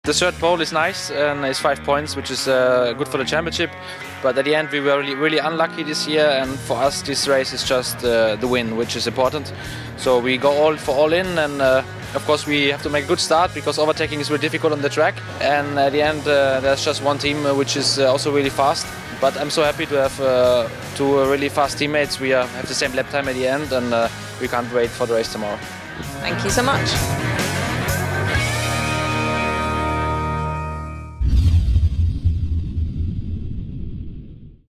6 Hours of Most 2021 - Interview of Marvin Fritz after the YART pole position
🎙️ Marvin Fritz: "We were unlucky this year, so now it's all in for the race victory."